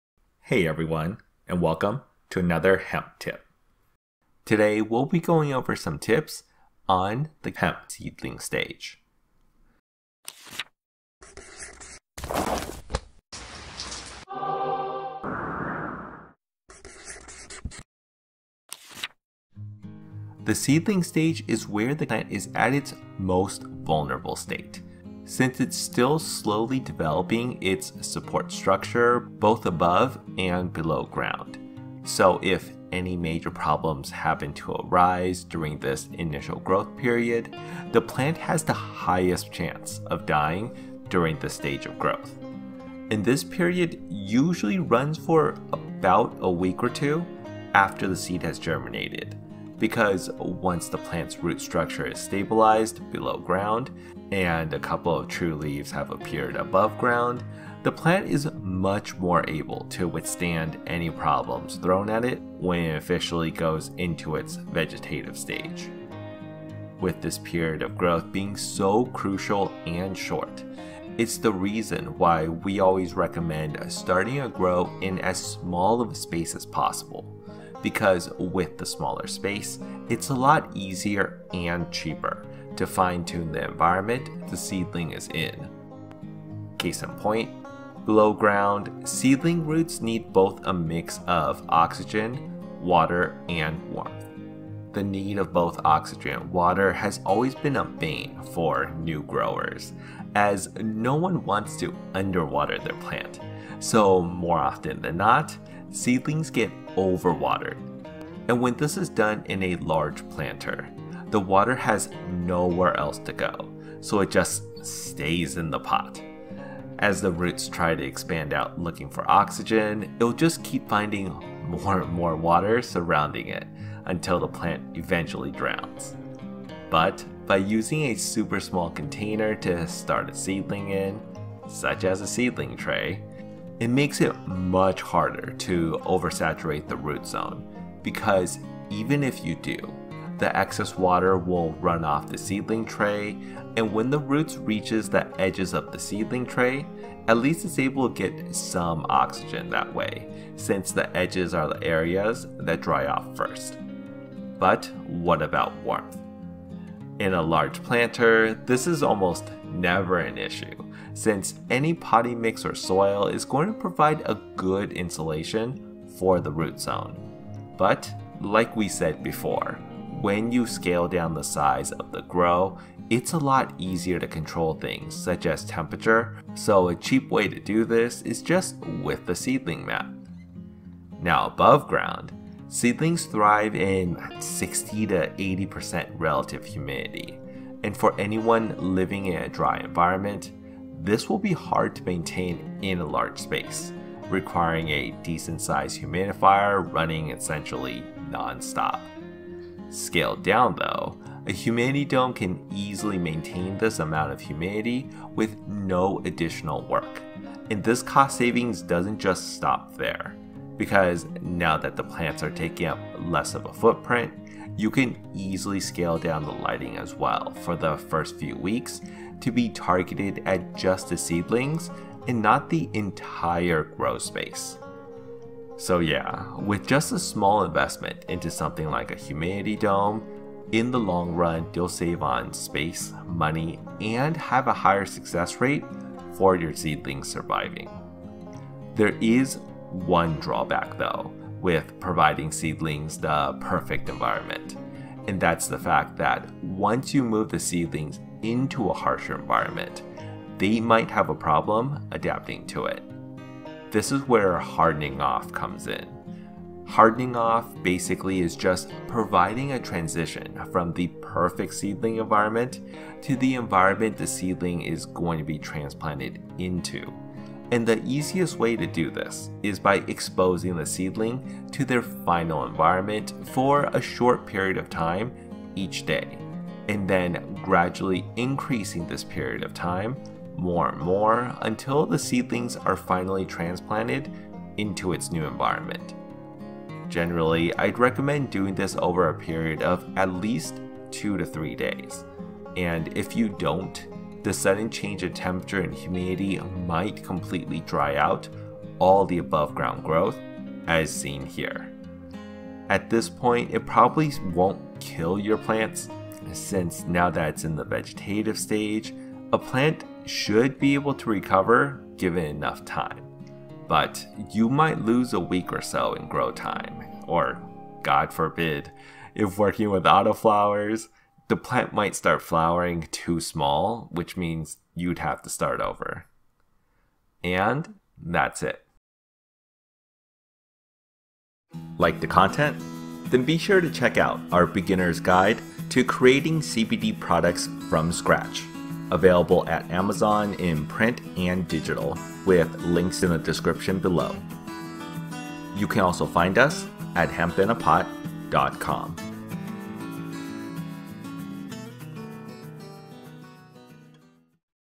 Tips for Hemp Seedling Stage
Keep your hemp seedlings alive with these tips and tricks!